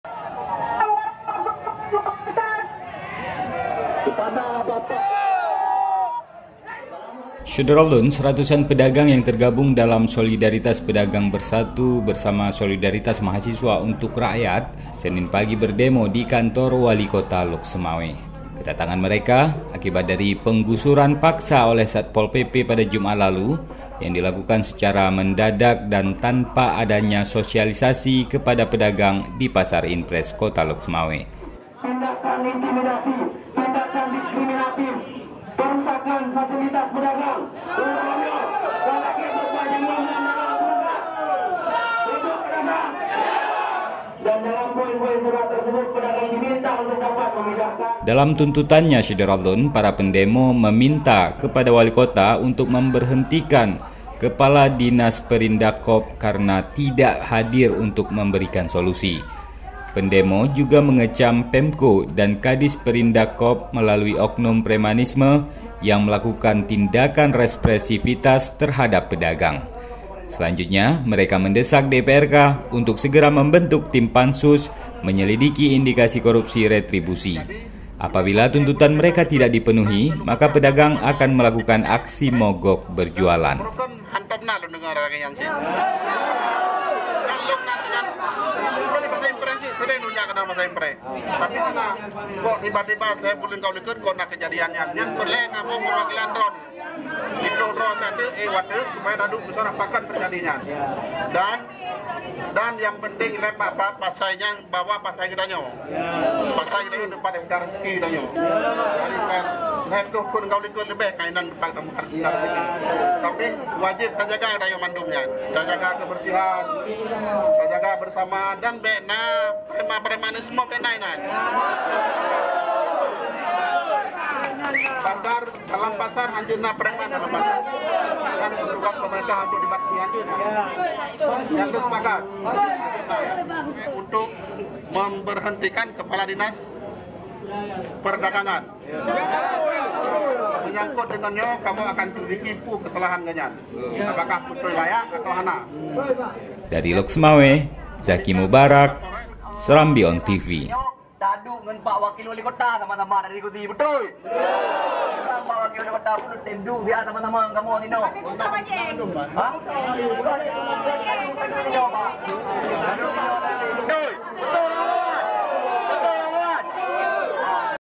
Lapak Digusur, Pedagang Pasar Inpres Demo ke Kantor Wali Kota Lhokseumawe
SERAMBITV.COM, LHOKSEUMAWE - Seratusan pedagang yang tergabung dalam Solidaritas Pedagang Bersatu (SPB) bersama Solidaritas Mahasiswa untuk Rakyat (SMuR), Senin (16/9/2019) pagi berdemo di kantor Wali Kota Lhokseumawe.

Mereka mengecam Pemerintah Kota Lhokseumawe dan Kepala Dinas Perindagkop melalui oknum premanisme yang melakukan tindakan represifitas terhadap pedagang Pasar Inpres Kota Lhokseumawe.

Koordinator Aksi, Dedi Ismatullah, dalam pernyataan sikap menjelaskan, berawal dari penggusuran paksa oleh Satpol PP pada Jumat (30/08/2019) di Pasar Inpres.

Menurut pedagang, penggusuran tersebut dilakukan secara tiba-tiba dan tanpa adanya sosialisasi. Sehingga pada Senin (2/9/2019) sore, para pedagang bersama SMuR mendatangi Disperindagkop untuk menyampaikan aspirasi dengan harapan adanya solusi.

Namun, Pemko Lhokseumawe melalui Disperindagkop cenderung menyudutkan pedagang yang diminta untuk dapat membongkar lapak berjualan di pasar.

Dalam tuntutannya, para pendemo meminta kepada Wali Kota untuk memberhentikan Kepala Dinas Perindagkop karena tidak bisa hadir untuk memberikan solusi.

Mereka juga mendesak DPRK untuk segera membentuk tim pansus untuk menyelidiki indikasi korupsi retribusi di Pasar Inpres Lhokseumawe.

Apabila tuntutan mereka tidak dipenuhi, maka pedagang akan melakukan aksi mogok berjualan.